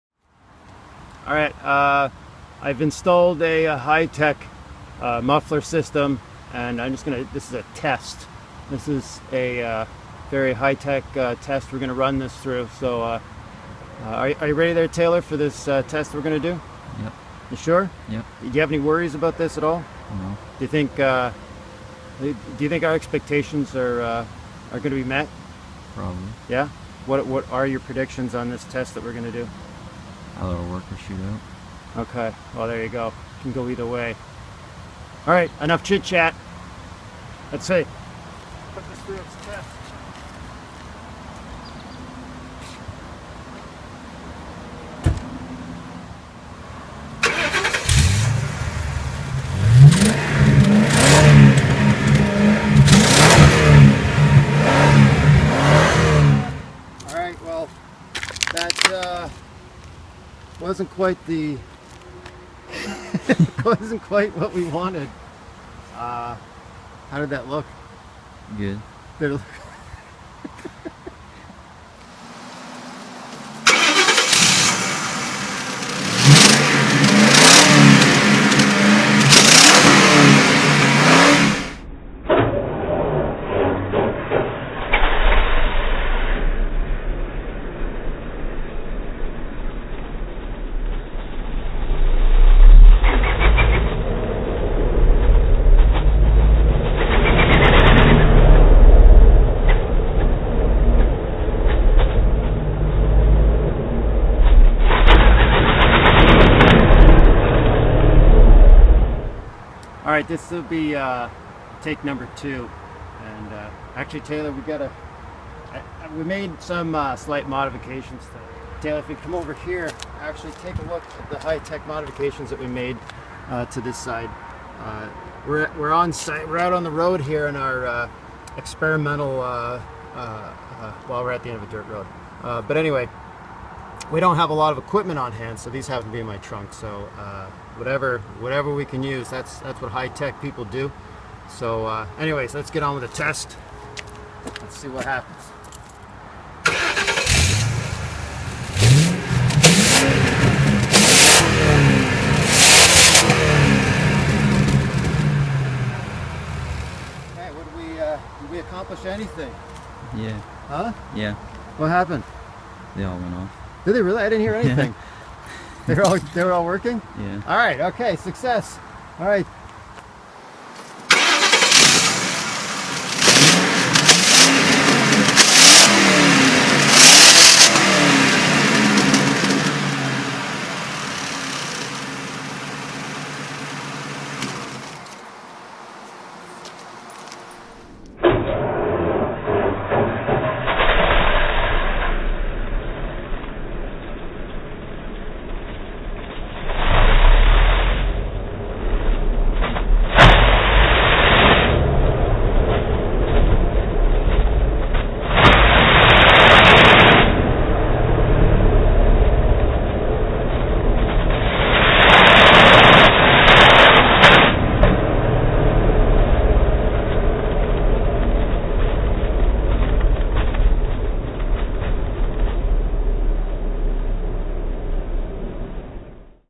Hightech muffler upgrade, test 1 and 2
I saw somebody do this and I thought...gotta give this a try.  Grabbed what I though were "noise makers" at the dollar store but as you see in the video, they make no noise. It was a lot of fun anyway, check out the other video where I do the same thing but with whoopee cushions. WHAT HAPPENS???